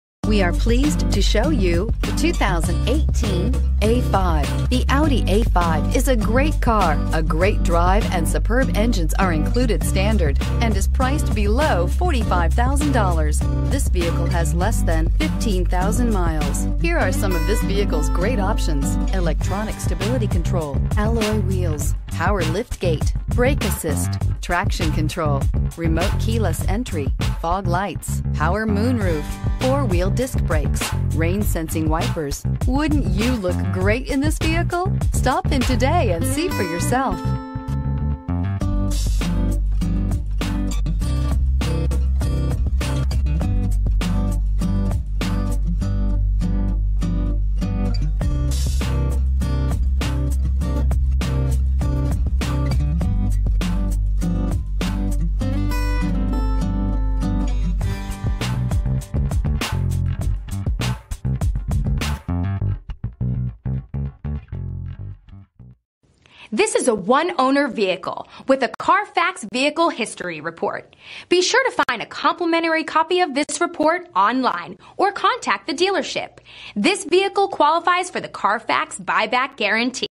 2018 Audi A5 2018 Audi A5 2.0T Premium Plus FOR SALE in Bakersfield, CA L1327
2018 Audi A5 2.0T Premium Plus

quattro, Nougat Brown w/Leather Seating Surfaces, ABS brakes, Alloy wheels, Compass, Electronic Stability Control, Front Bucket Seats, Front Center Armrest, Front dual zone A/C, Heated door mirrors, High intensity discharge headlights: Bi-xenon, Illuminated entry, Power Liftgate, Power moonroof, Power passenger seat, Remote keyless entry, Split folding rear seat, Traction control. CARFAX One-Owner. Mythos Black Metallic/Black Roof  Recent Arrival! Odometer is 1433 miles below market average! 24/34 City/Highway MPG  Vehicle price and availability are subject to change without notice.